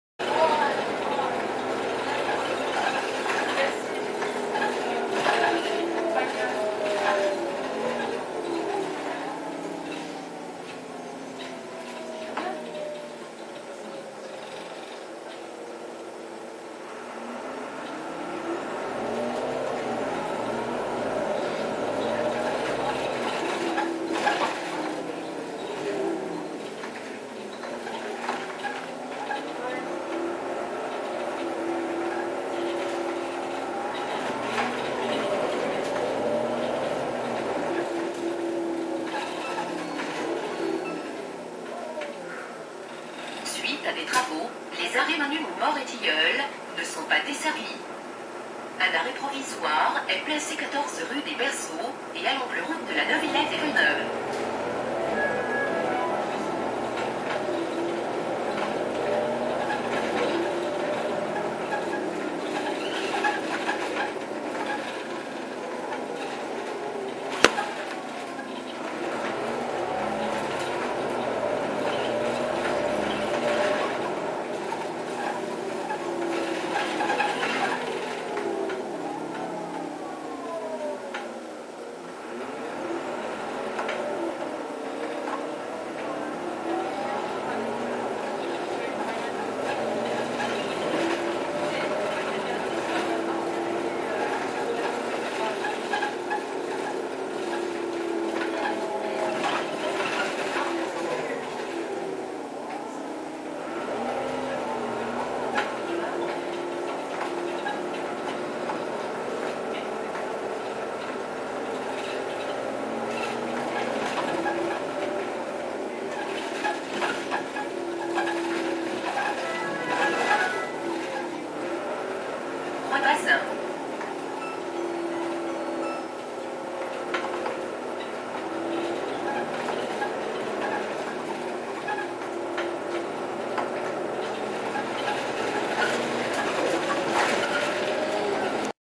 Citura Déviation entre Tilleuls et Croix Bazin (ligne 14)
Le Jeudi 4 et le Vendredi 5 Mars 2021, la ligne 14 a été déviée entre les arrêts Tilleuls et Croix Bazin vers La Neuvillette Mairie, et de Croix Bazin à Farman en direction de Sébastopol. Vidéo réalisée le Jeudi 4 Mars 2021, à bord du Heuliez GX317 n° 256.